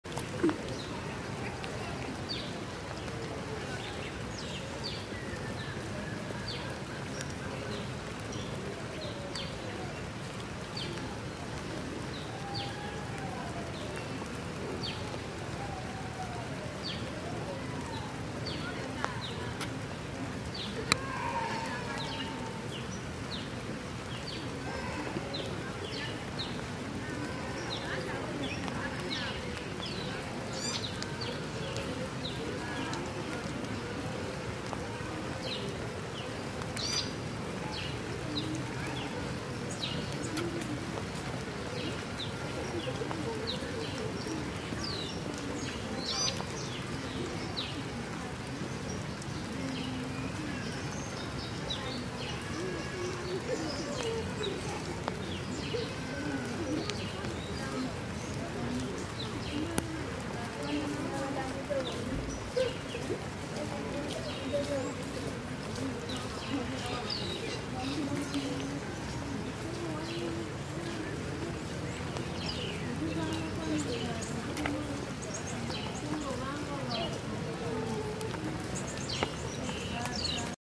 Tiny Cute Blue Birds Feeding Under Rain#birds
Tiny cute blue birds, feeding under rain🥰